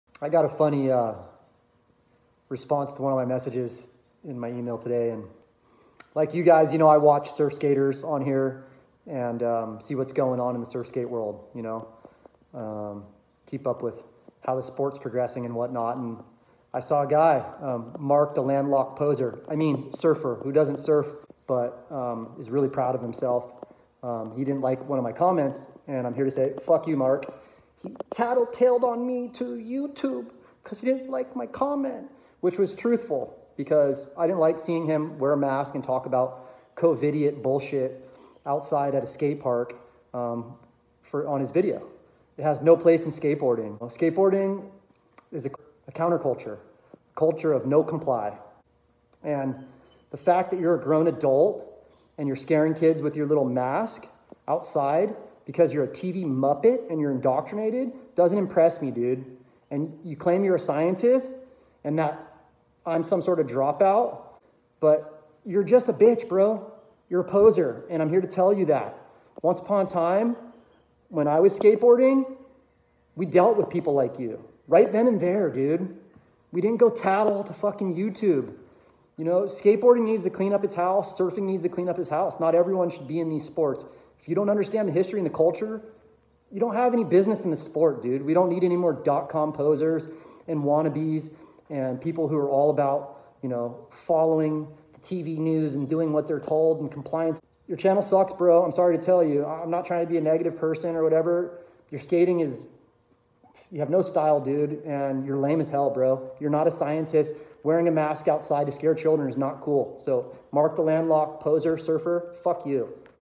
Please read Description below.
I’m sick of posers and wannabes in surfing and skating. THERE IT’S SAID!! This isn’t the same as one learning a sport, that’s different. BUT!!, Surfing and Skating are tribes, you don’t just get to be in either because it’s cool right now for pros making money to sell it to you. Before the sellouts of both industries helped ruin the sports, they know damn well there were historic rights of passage, and that these sports were counter culture sports that went against the mainstream grain, NOT WITH IT! They weren’t for everyone. When I was a kid skating wasn’t cool. Period. We earned respect by saying fuck you to the gangsters and throwing blows if needed. We didn’t let compliant wannabes into our tribe because woke weenies thought they were entitled to entry and making up new rules. What’s my issue exactly you ask? Lately, surfing, I get people telling me, on small days, that my local break, “is a beginner spot”; that’s code for: I don’t know shit about surfing, don’t care, and am gonna be the kook that shoots my board at your head from the shoulder cause the Internet told me this spot was safe for me. Nah MFr, it don’t work like that! There may be weak people online protecting other weak people to create community standards for a new weak class of enslaved nerds in the meta, but the ocean spits out the weak and the locals are sick of being shit upon in the real world by thumb warriors who wear masks behind their phone screens to feign woke virtue￼. Same goes for skating. If you need a face mask to skate outside around kids, you don’t need a skateboard, you need a diaper and someone to wipe your ass for you. Danger and risk are inherent in skating and surfing (and in life), and though I wish all all the best when they’re out practicing sport, truly!!, both sports are far more dangerous than the imaginary Pharma lies you heard on TV and bought into because of your relatively low IQ’s. Stop skating and surfing, go seek shelter at home as your masters tell you! PS, I’m not here for followers, or likes, you can do that voluntarily if you like, but I’ll never ask nor suggest. I don’t care either way, unlike Mark, if 5,000 people follow me, there’s no prize for a lucky follower. This ain’t about popularity (how many nerds I can’t get to follow me) or money. This is about skating, nostalgia for the real skate/surf tribe vibes, and getting people to return to their roots, PRECISELY BECAUSE WE NEED LESS SLAVE PROGRAMED ROBOTS, and MORE REBELLIOUS, NONCOMPLIANT, ARTISTS!!!, TO RETURN TO EARTH. I’m done bitting my lip. I don’t have to be nice. Fuck Meta, Zuck, Gates, Ass-Schwab, and all the muppets backing NWO slavery via their compliance to lies and willingness to remain ignorant because it seems most popular currently.